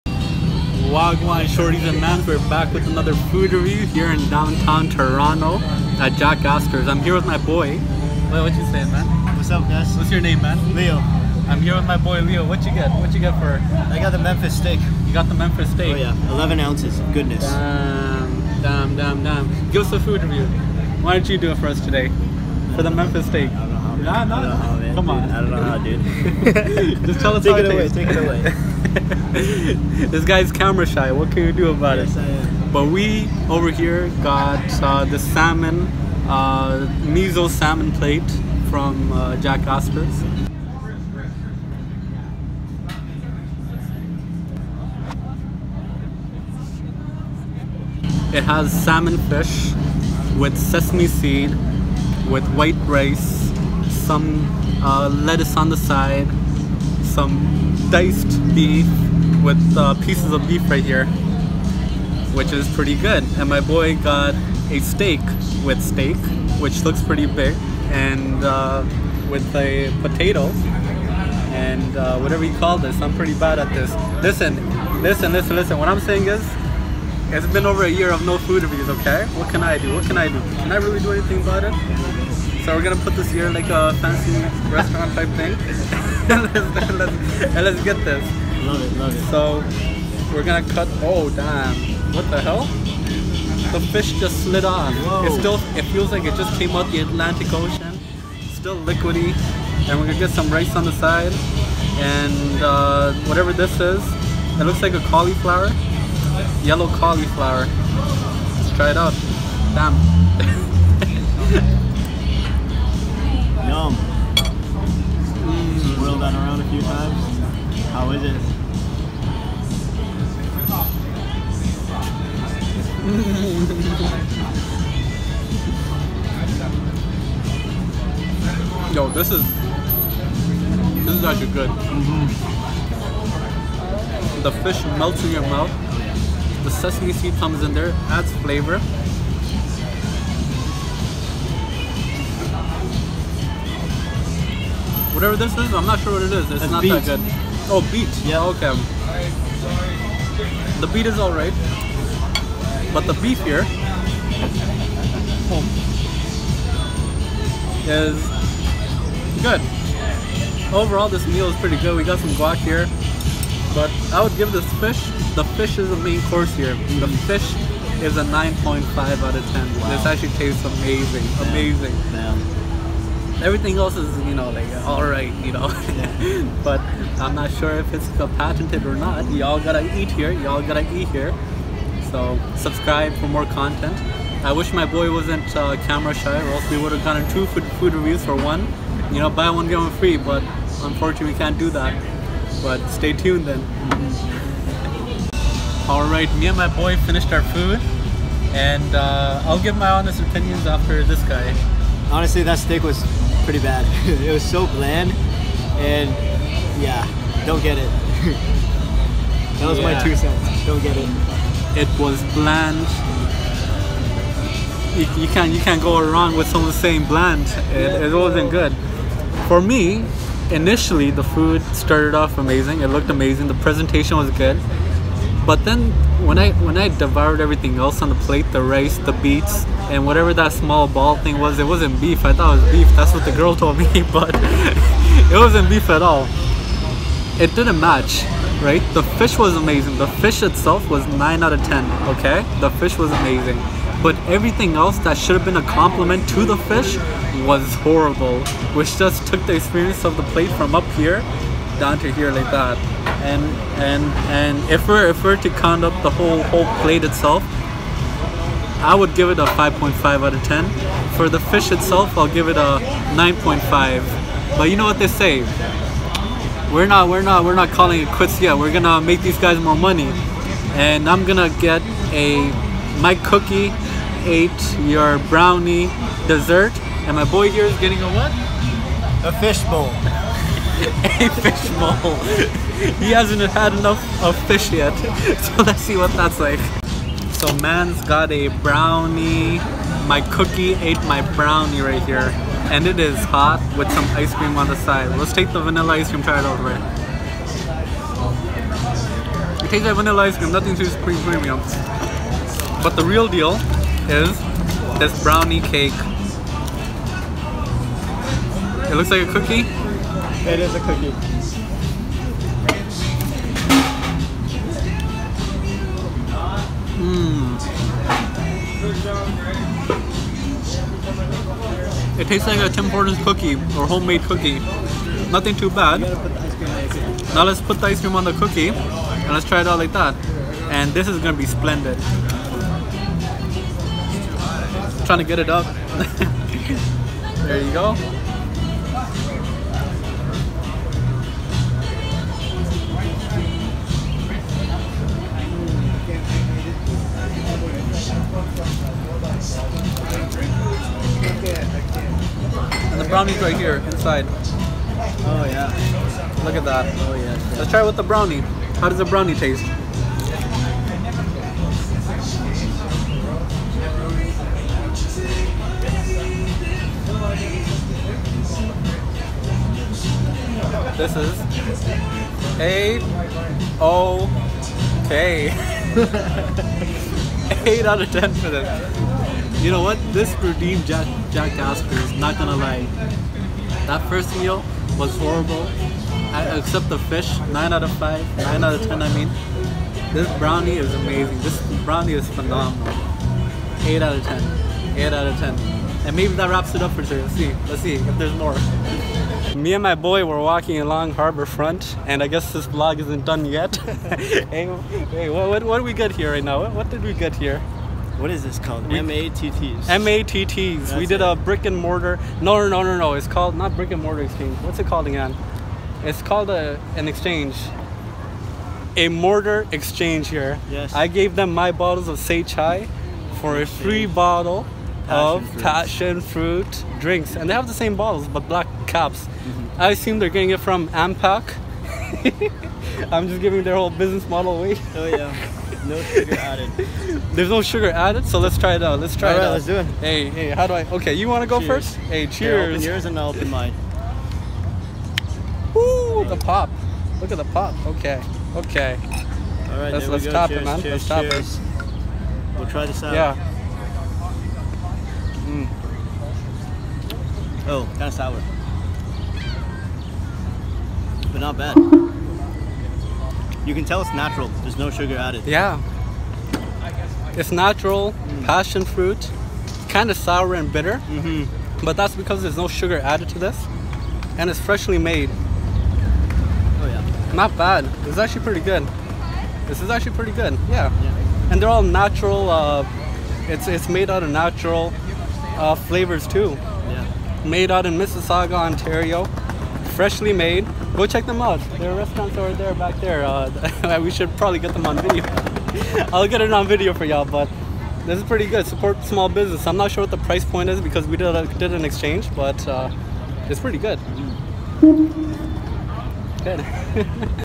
Jack Astors and More Food Review
Today we hit up Downtown Toronto to eat out on a patio and enjoy some drinks at Harbour Front in Downtown Toronto.